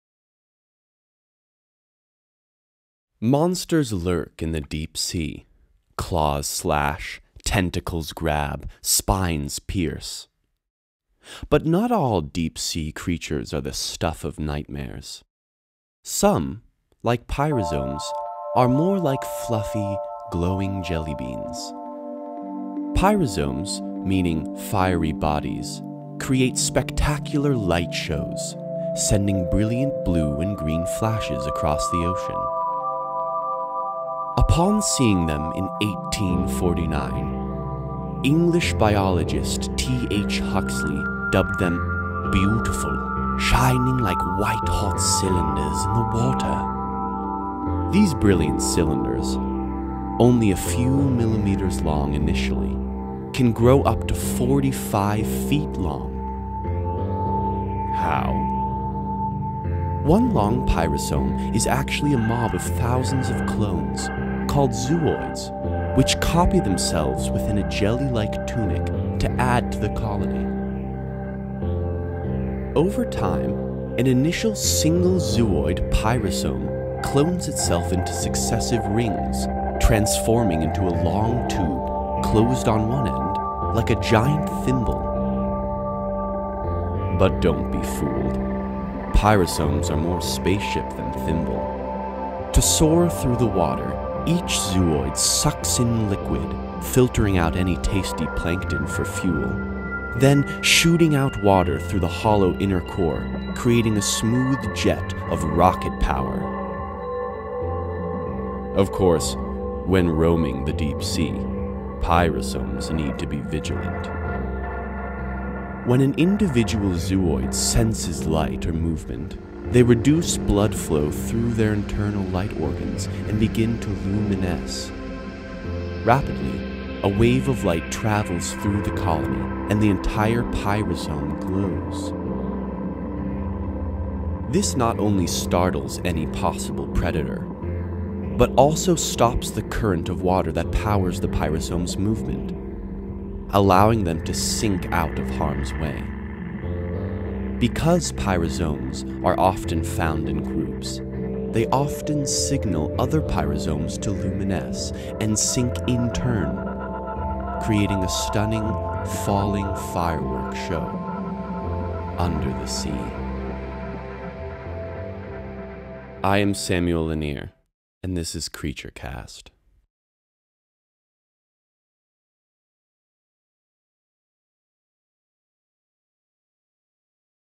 CreatureCast - Pyrosomes
Anna Zeidman introduces pyrosomes, "fiery bodies" that create brilliant underwater light shows and grow into giant swimming colonies.

This episode of CreatureCast was narrated by Samuel Lanier. The music is by Lee Rosevere,